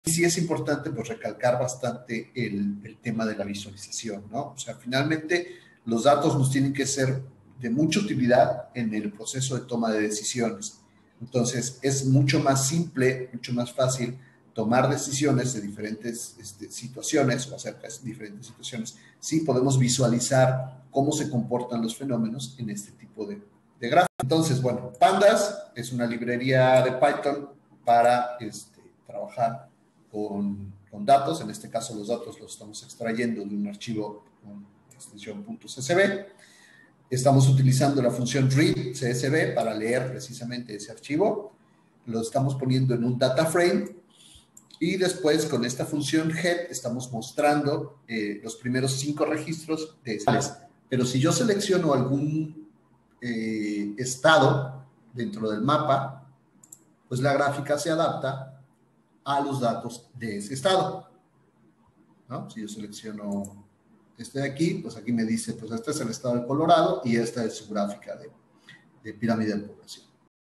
Trailer de taller: amChart y Python para desarrollo de plataformas de visualización
Es una muestra del taller que se impartió en la Escuela de Verano CentroGeo 2021. Taller impartido por el Dr. Gandhi Hernández.